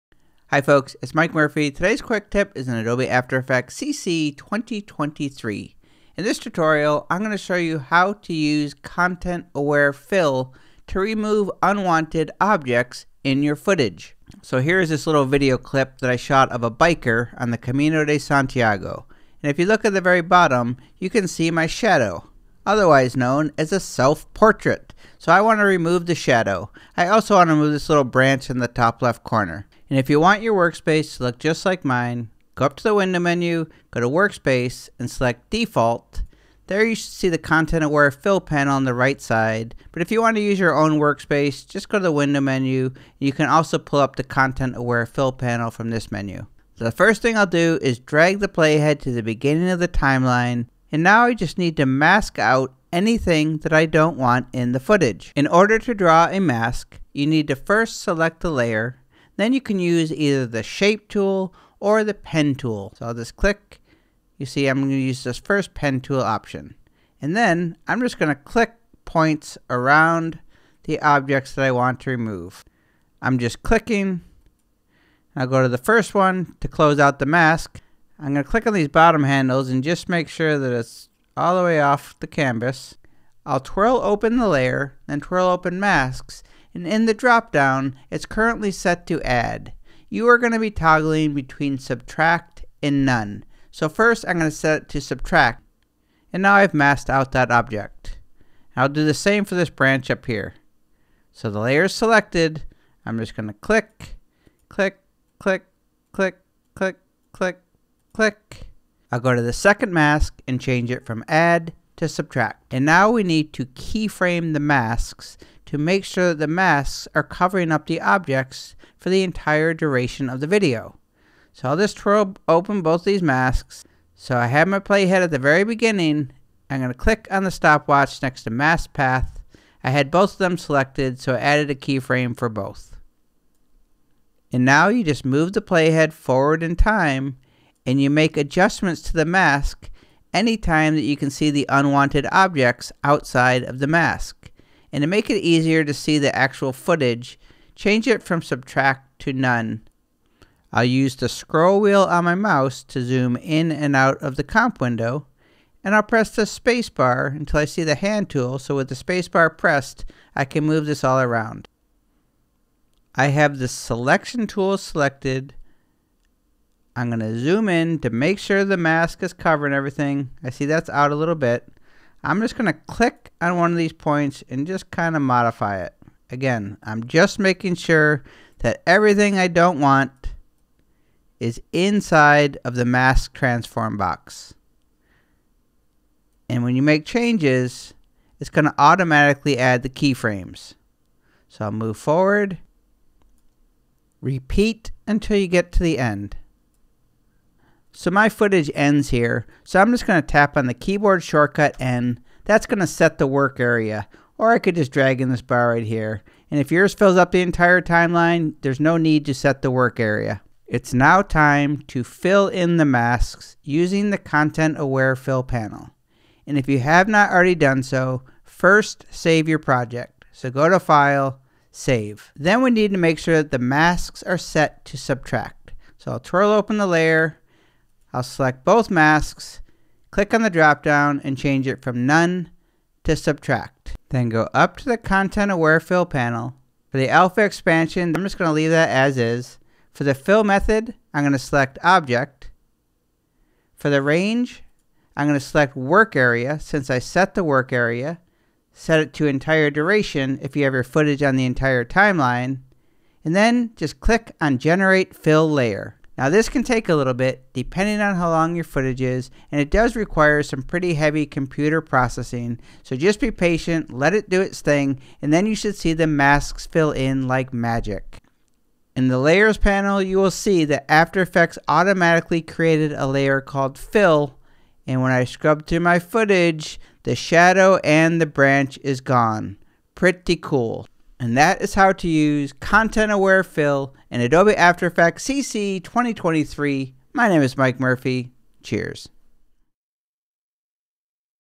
After Effects: How To Use Content-Aware Fill
After Effects CC 2023: How To Use Content-Aware Fill

This tutorial will show you how to use Content-Aware Adobe After Effects CC 2023 to remove unwanted objects in your footage.

Note: If Content-Aware Fill does not ‘fill’ in the masked areas, make sure your masks are set to ‘Subtract’ before clicking ‘Generate Fill Layer’

Workspace Used in Tutorial:Default

Keyboard Shortcut:V: Select Tool
M: Mask
MM: Reveal All Masks
N: Set End of Work Area
Cmd/Ctrl + S: Save Project

Chapters:
00:00  Intro: About the Tutorial
00:29  Default Workspace: Content-Aware Fill Panel
00:46  Mask Unwanted Objects
01:02  Pen Tool (G): Create Masks
01:22  Mask Property: Change to Subtract
01:52  Animate (keyframe) Mask
02:29  Change from 'Subtract' to 'None'
03:30  Content-Aware Fill Panel (Fill in Masks)

How To Use Content-Aware Fill:
1. Set Workspace to Default (or use Window…Content-Aware Fill to show Panel)
2. Create New Composition (Cmd/Ctrl + N)
3. Drag video footage on Composition
4. Select the Mask Tool (G)
5. Click around object that you want removed from footage
6. Close out mask
7. Twirl open layer
8. Twirl open Mask
9. Click drop-down and change from ‘Add’ to ‘Subtract’ to reveal layer below (or transparency)
10. Repeat for any other objects to remove from footage

How To Animate Masks:
1. Drag Play-head to the beginning of timeline
2. Twirl open Layers
3. Twirl open Masks
4. Click on stopwatch next to ‘Mask Path’ property to add keyframes
5. Change Masks from ‘Subtract’ to ‘None’ so it is easier to see the unwanted objects
6. Move forward 1 frame at a time (press Cmd/Ctrl + Arrow)
7. Tap ‘V’ for Selection Tool
8. Adjust masks if the unwanted object moves outside of the masked area
9. Repeat to the end of the footage (keyframes will be added for each new adjustment)

How To Use Content-Aware Fill Panel To Fill in Masks:
1. Save Project
2. Twirl open layer
3. Twirl open Mask
4. Click drop-down and change from ‘None’ to ‘Subtract’
5. Go to Content-Aware Fill Panel
6. Change Fill Method to ‘Object’
7. Change Range to ‘Work Area’ (keep at Entire Duration if you did not set Work Area)
8. Click ‘Generate Fill Layer’
9. Wait for AE to analyze footage
10. Masks should fill in like magic!
11. Drag play-head to beginning and press Spacebar to preview